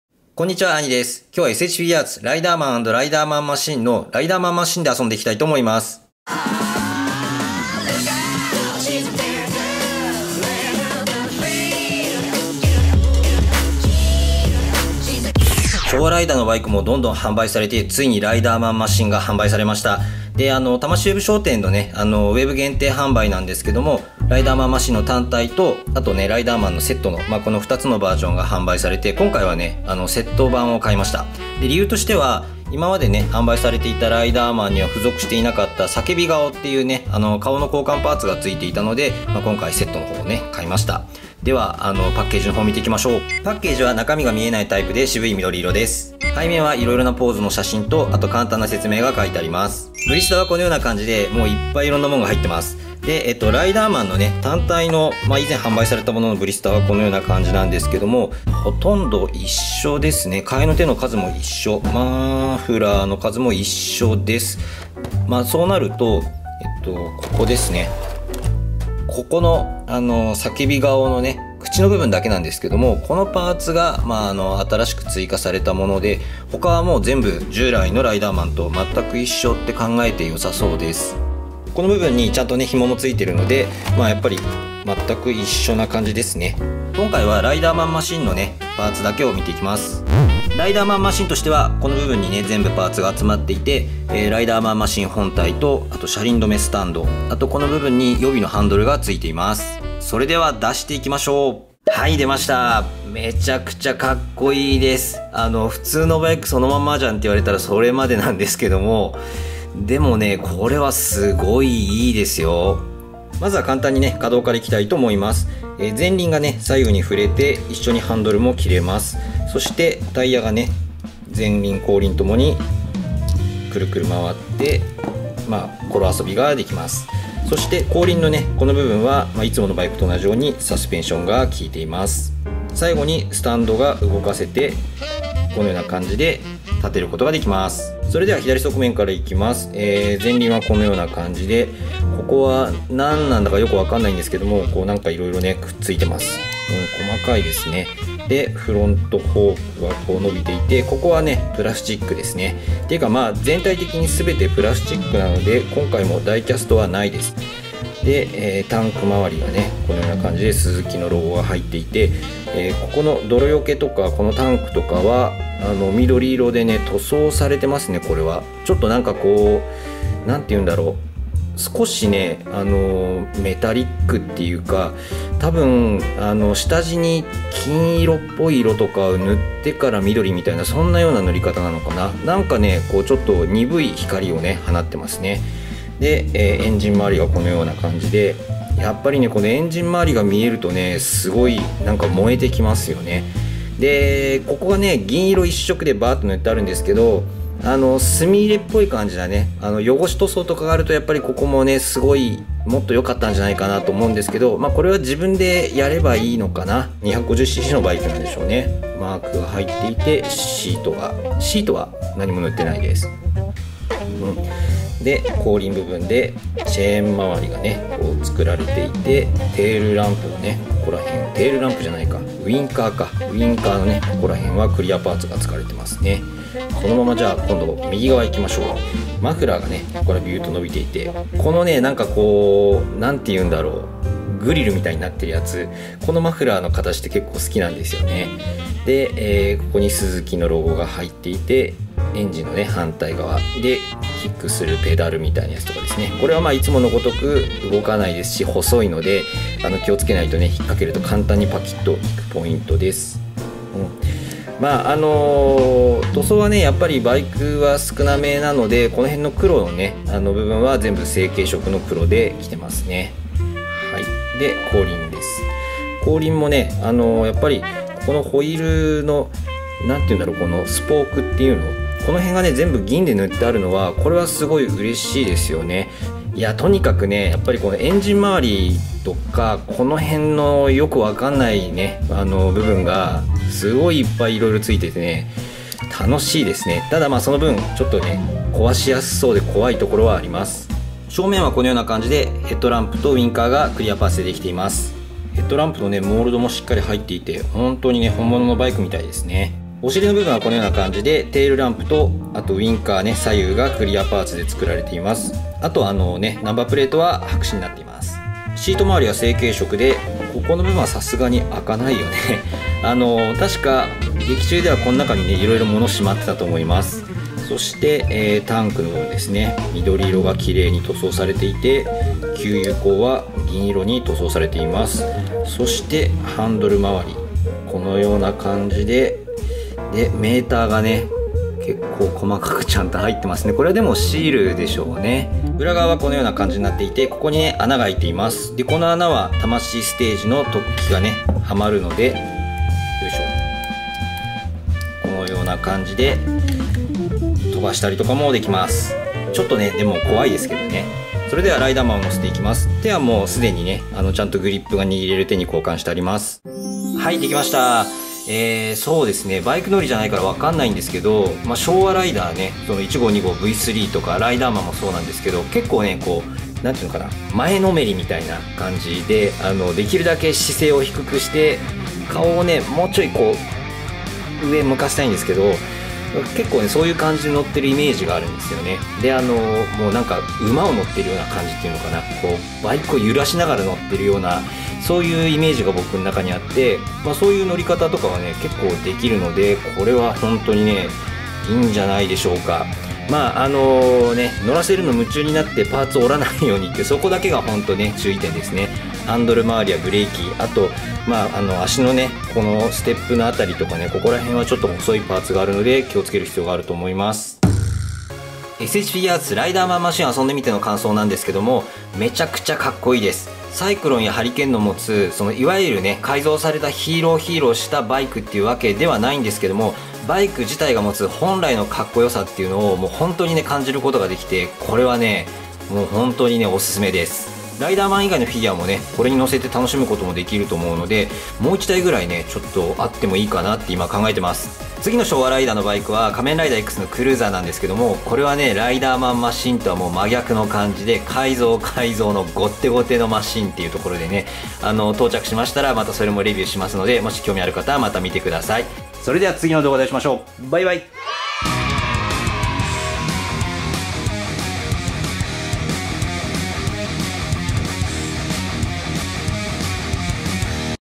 【S.H.Figuarts】 復讐の鬼 ライダーマンマシン レビュー ★★S.H.フィギュアーツ 仮面ライダー ドライブ kamenrider riderman machine
★★★【アニのゲーム実況チャンネル】アニゲー大作戦★★★
クーリッシュのCM：改造人間NOZOMINが最近ツボです。

とりあえずフィギュアーツ タイプワイルド タイプテクニックは買おうと思います。
フィギュアーツ トライドロンはどうしたら良いのだろう...